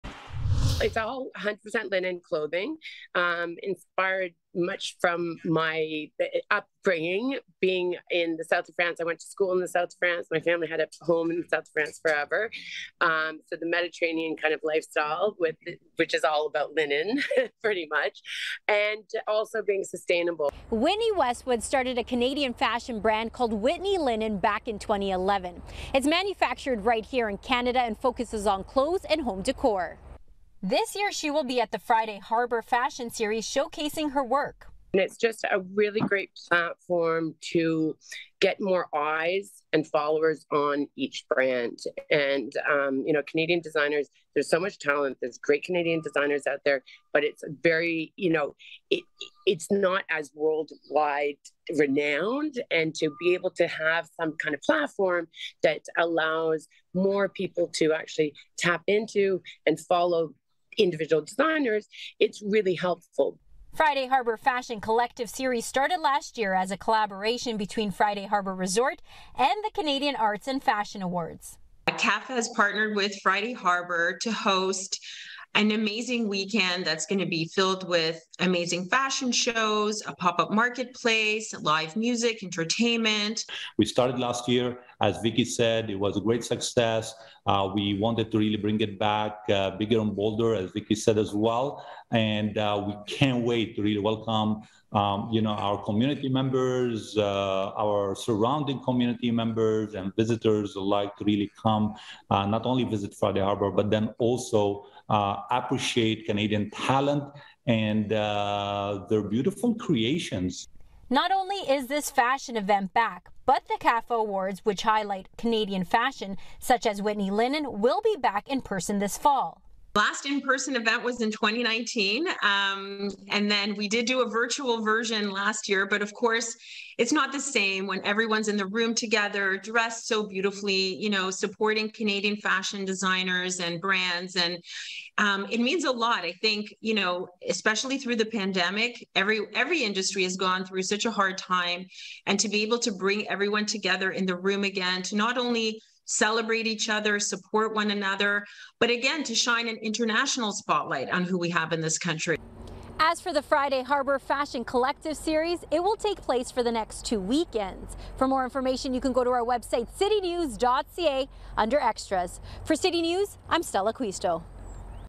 Bringing high fashion lakeside
Friday Harbour Fashion Collective Series is back and hitting the runway is Canadian designer Whitney Linen.  Stella Acquisto reports.